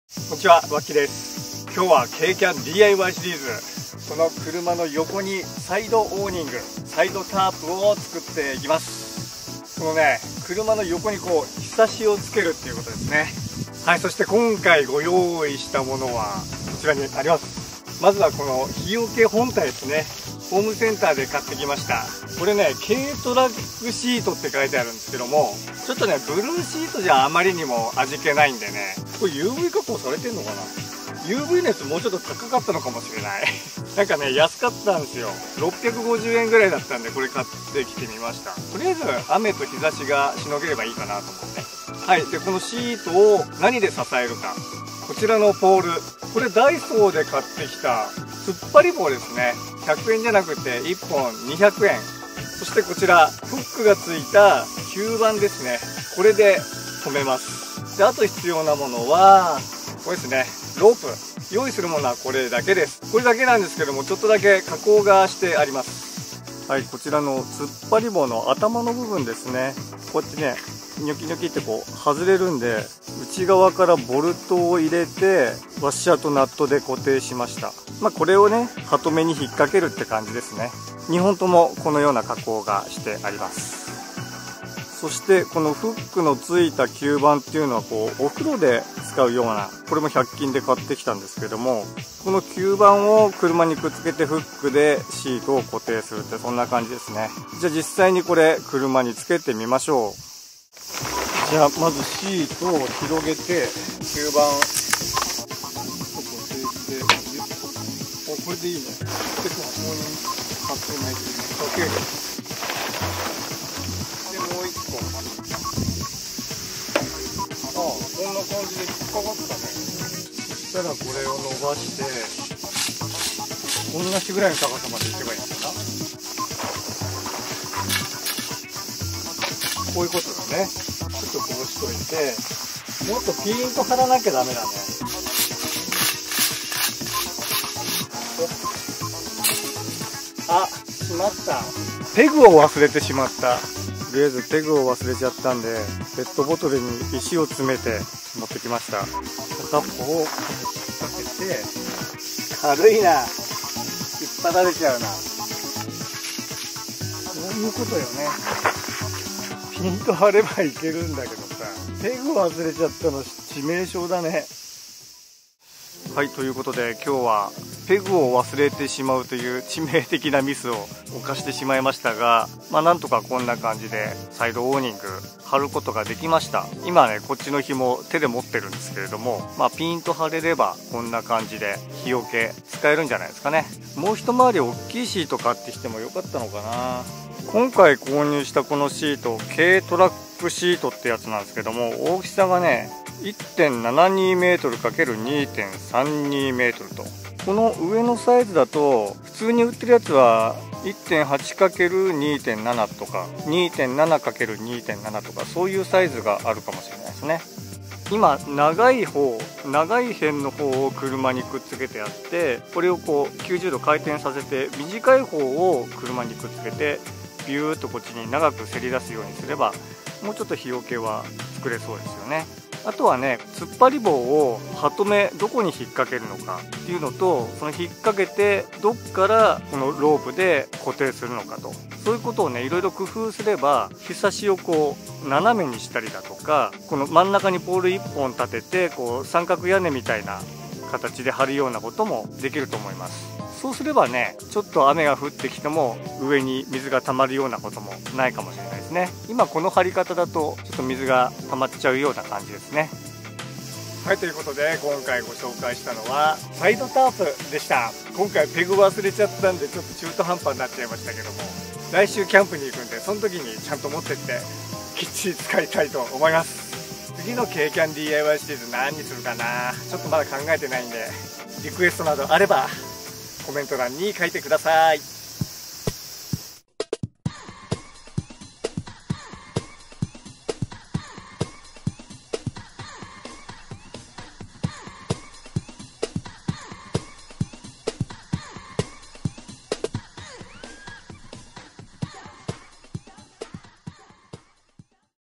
【軽キャンDIY】#08「サイドオーニング」
今回は安い材料を使って「サイドオーニング」を作ってみました。がしかし…ペグを忘れてちゃんと固定できないという…(^^;
でもこれオートキャンプに使えそうです♪
▼説明欄要チェック♪

【ベース車両を購入したお店はこちら】
「YouTubeを見た」と言ってご成約された方には、プレミアムサービスがあるそうです♪(^^)
▶GET106